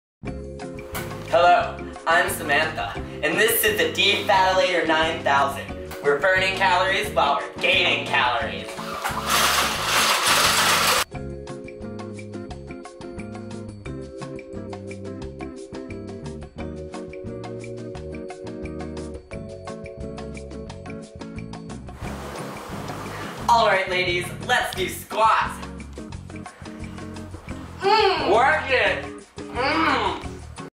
Sharkcano - Item #3
A modified stationary exercise bicycle that whips heavy cream into whipped cream. (don't forget the late 80s aerobics wear)